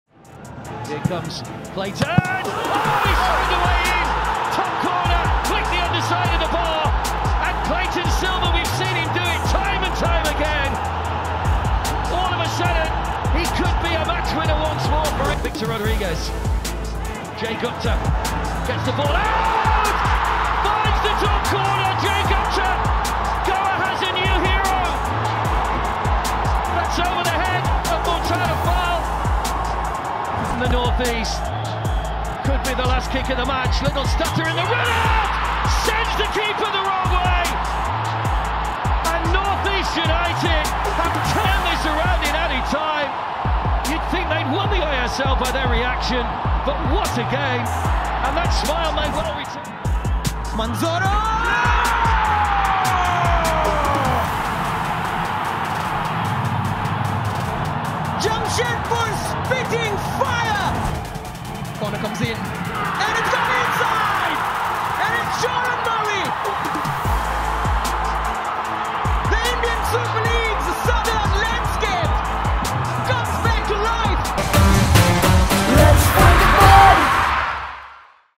Scenes at the Death! 🤩 | Stoppage Time Winners in ISL 2023-24
𝐋𝐚𝐭𝐞 𝐖𝐢𝐧𝐧𝐞𝐫𝐬 𝐦𝐚𝐤𝐞 𝐟𝐨𝐫 𝐆𝐫𝐞𝐚𝐭 𝐌𝐞𝐦𝐨𝐫𝐢𝐞𝐬! 🤩

Contents of the video:
0:00- Cleiton Silva's Goal vs HFC
0:14- Jay Gupta's Goal vs OFC
0:30- Ibson Melo's Goal vs JFC
0:49- Jeremy Manzorro's Goal vs EBFC
1:00- Jordan Murray's Goal vs OFC